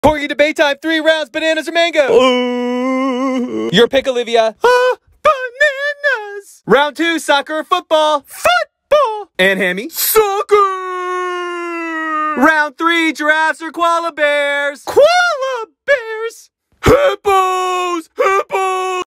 Talking corgis debate each other | Hammy and Olivia
The talking dogs have to debate for three rounds. Which corgi do you side with the most? Which dogs choices do you agree with?